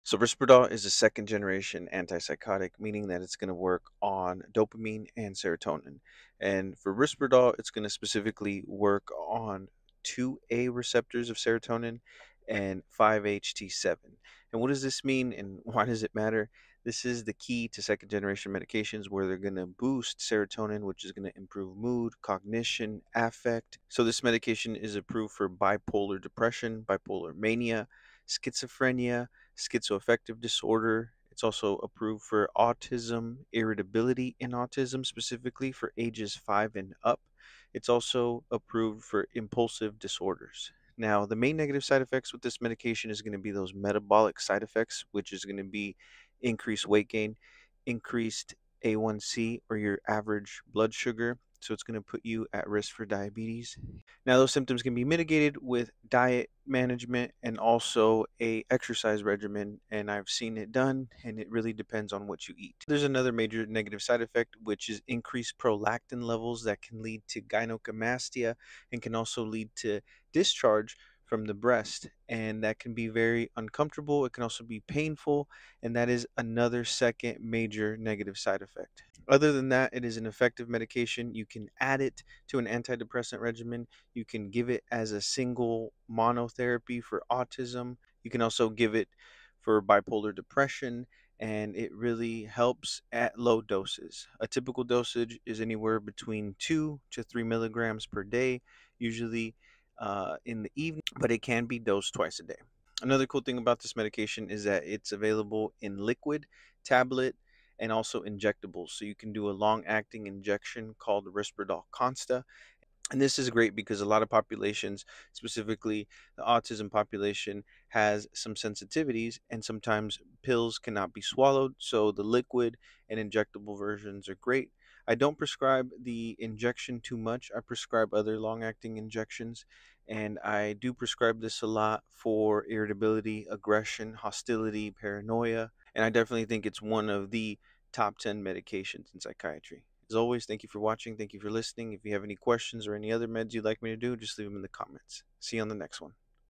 Risperidone (Risperdal): Understanding a Second-Generation Antipsychotic
As a psychiatric nurse practitioner, I often prescribe risperidone, a widely used second-generation antipsychotic that plays an important role in managing conditions such as schizophrenia, bipolar disorder, and irritability associated with autism. In this video, I’ll break down how risperidone works, its benefits, common side effects, and why its flexibility in formulation makes it such a valuable option in treatment.

One of risperidone’s strengths is the variety of ways it can be taken:

Tablets – the most common and convenient option for daily use.

Oral solution (liquid form) – a great alternative for those who may have difficulty swallowing pills.

Long-acting injectable (LAI) – provides steady medication coverage every two weeks, ideal for patients who prefer fewer daily medications or struggle with adherence.

By the end of this video, you’ll have a clear understanding of when and why risperidone may be prescribed, as well as what to expect if you or a loved one are starting treatment with it.